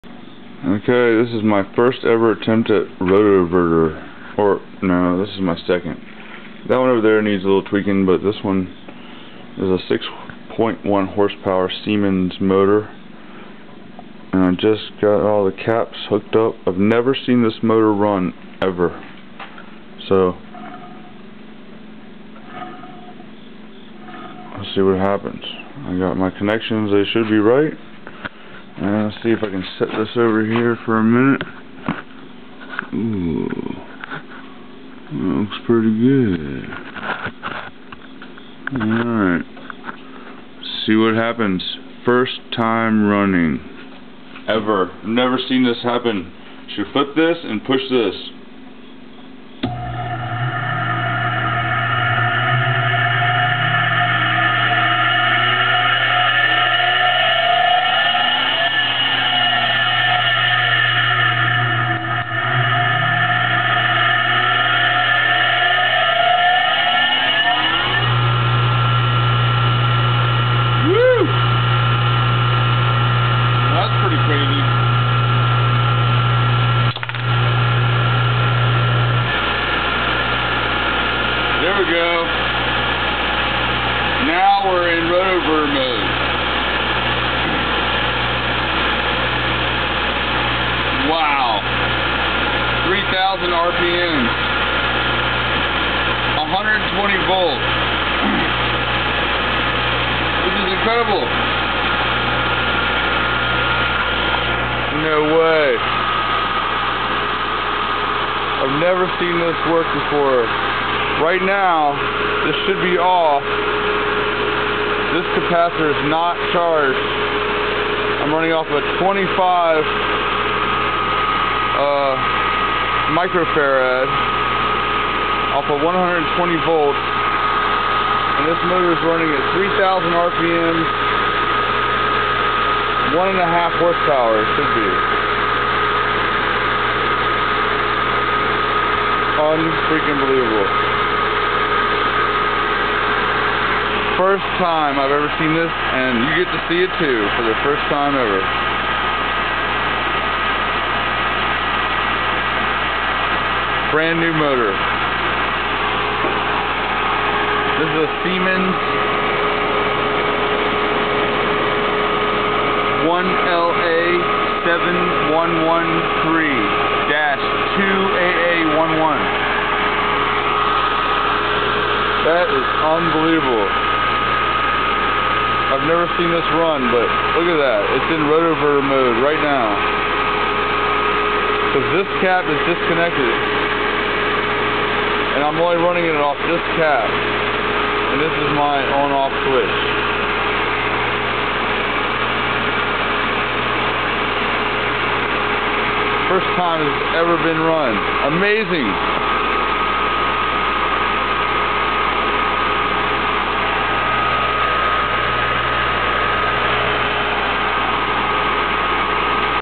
First Rotoverter attempt 460V 3 Phase, 6.1HP siemens motor
This was 6 years ago...  My first ever rotoverter... ;)